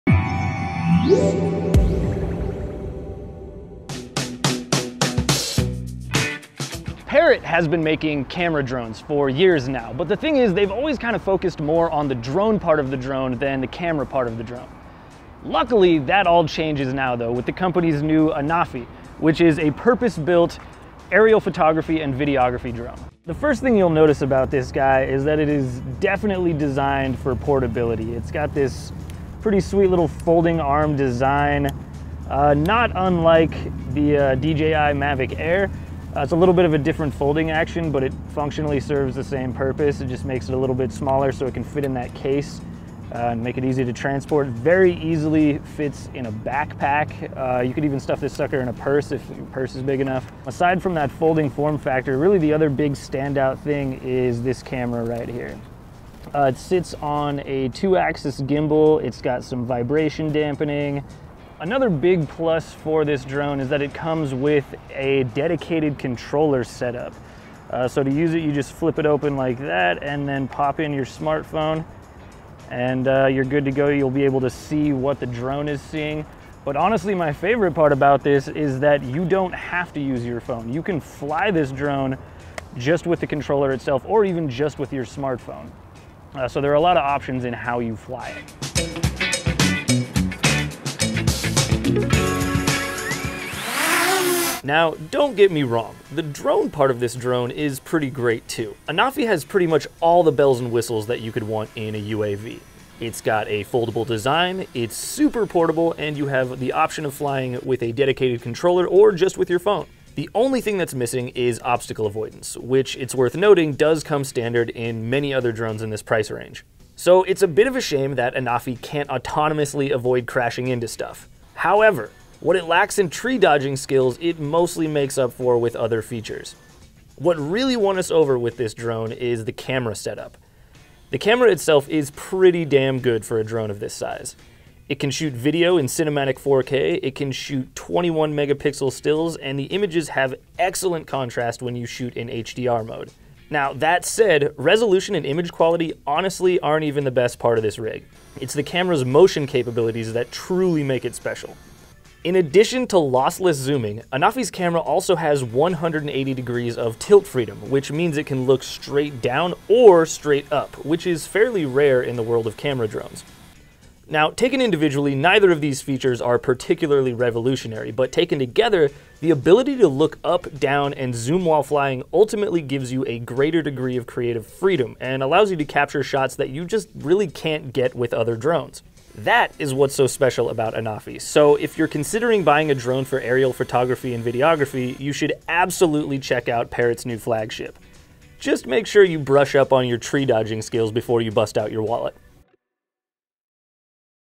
Parrot Anafi Drone - Hands On Review
Parrot's new Anafi camera drone features a compact, portable design, very similar to DJI’s Mavic drones. Anafi features hinged arms that fold letting the drone fit inside a pill shaped carry case.

The most notable feature on Anafi is its camera which can tilt 180 degrees and shoot straight up in the air. This is a 4K camera that shoots HDR video better shooting in high contrast scenes. It can also do lossless digital zooming.